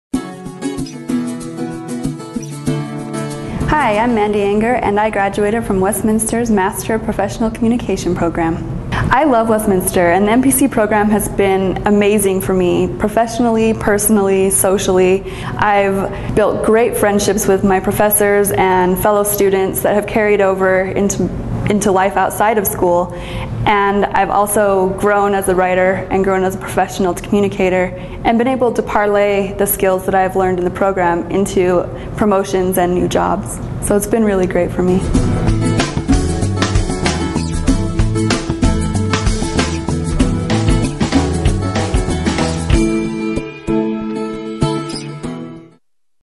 It's All About Mandy
Westminster graduate student, Mandy Anger,  tells why the Master of Professional Communication was a great fit for her.